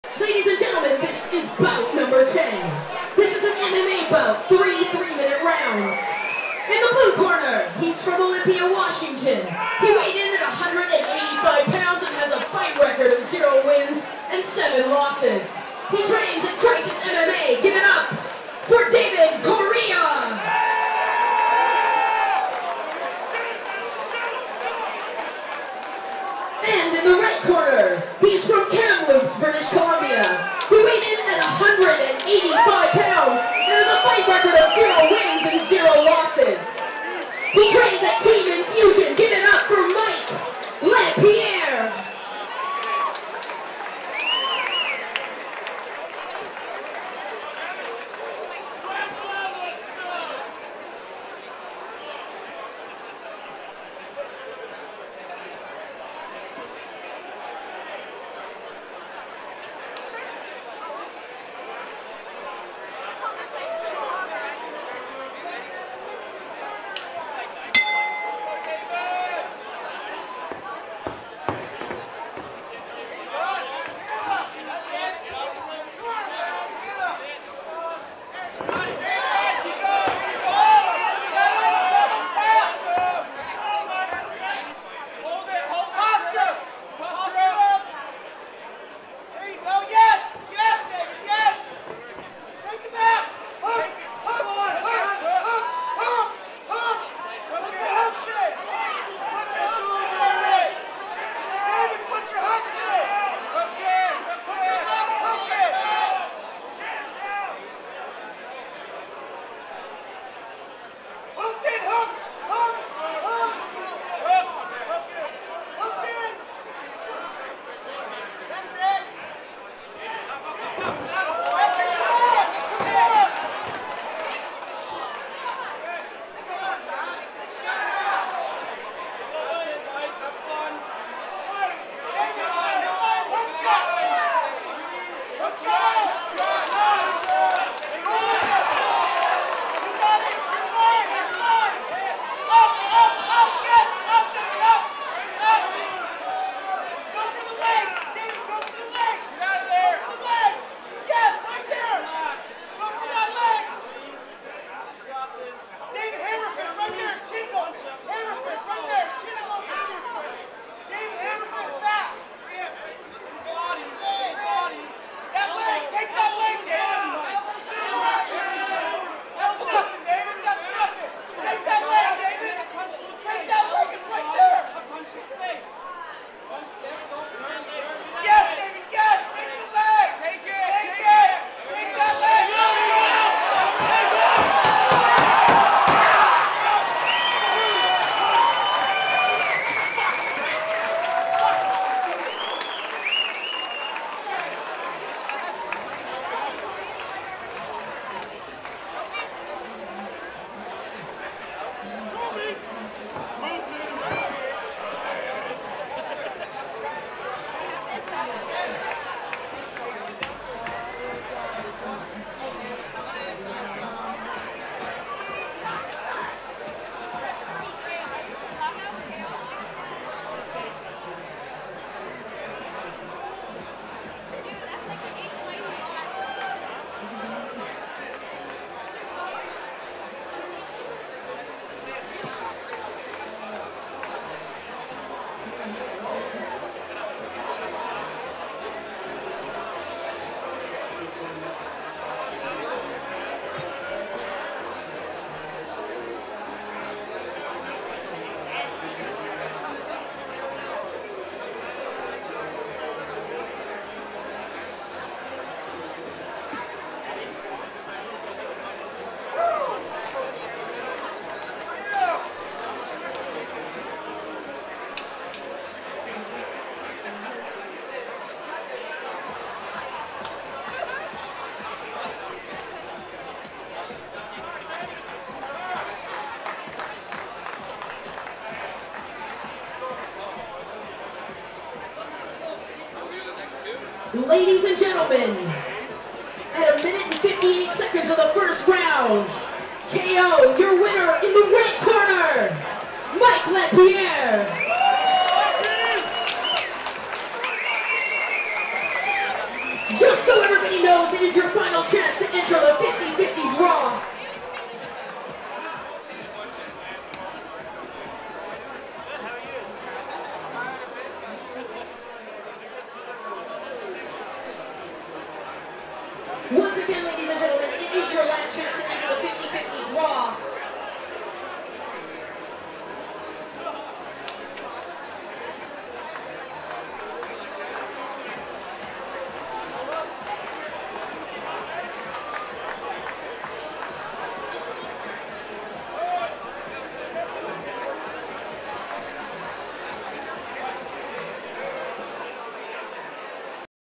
Total Mayhem II (May 4, 2012) - Fight #10 - Mike LePierre vs David Correa
Total Mayhem II (May 4, 2012) - Fight #10 (MMA - Middle Weight) - Mike LePierre (Team Infusion - Kamloops, BC) vs David Correa (Crisis MMA - Olympia, WA) A Gerry Gionco Promotion & An UnderDog Production. Jorge Parra (Videographer).